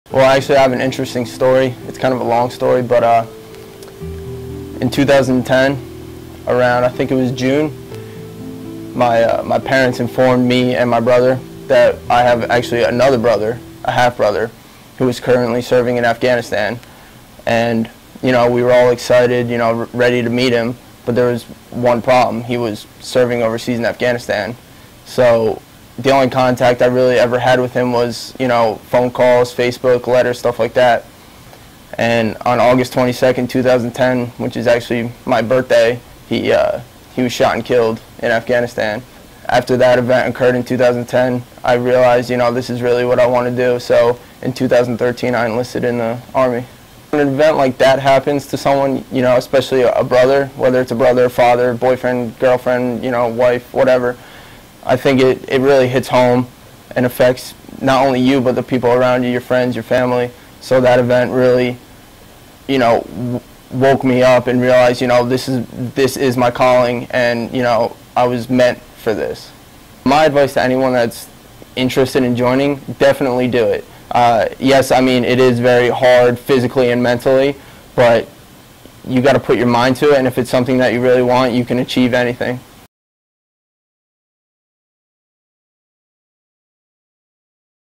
"Unique Brotherhood" - Nick Hussey's Story
MSU student and National Guard veteran Nick Hussey discusses his inspiration to join the military, following the loss of a brother he never had a chance to meet - after being shot and killed in Afghanistan.